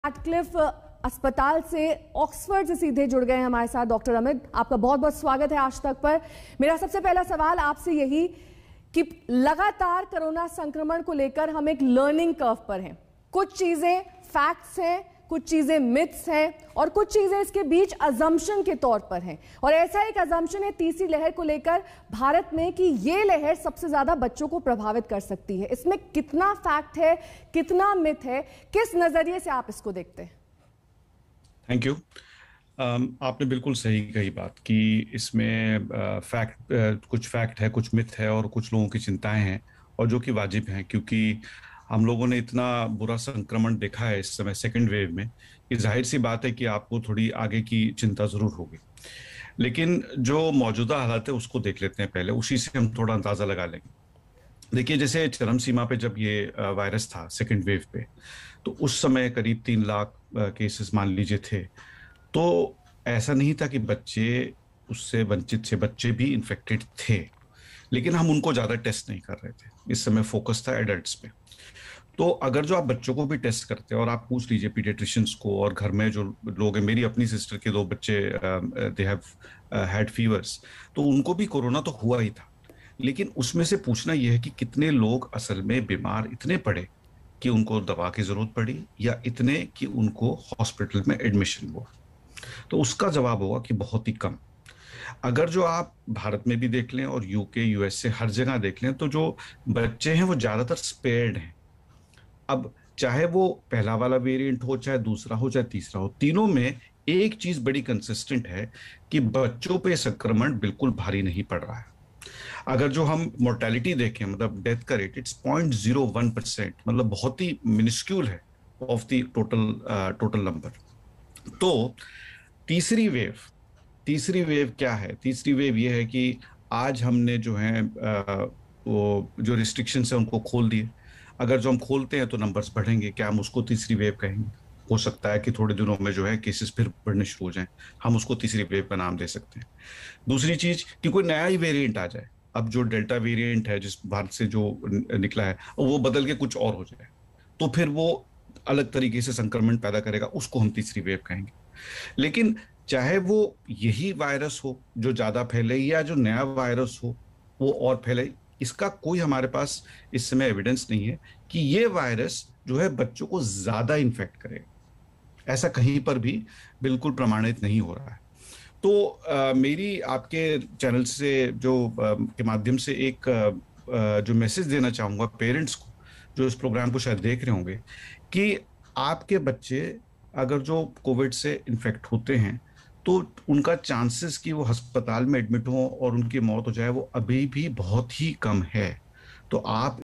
Corona की तीसरी लहर बच्चों को ज्यादा प्रभावित करेगी, इसमें कितना Fact है? Dr. Amit Gupta ने बताया
Corona की दूसरी लहर अब थोड़ी कम हुई है लेकिन लोगों को Corona की तीसरी लहर का डर सताने लगा है. अब सवाल ये है कि Corona की तीसरी लहर बच्चों को ज्यादा प्रभावित करेगी, इसमें कितना Fact है? देखें इस पूरे मामले पर ऑक्सफोर्ड के जॉन रेडक्लिफ हॉस्पिटल के Dr. Amit Gupta ने क्या बताया...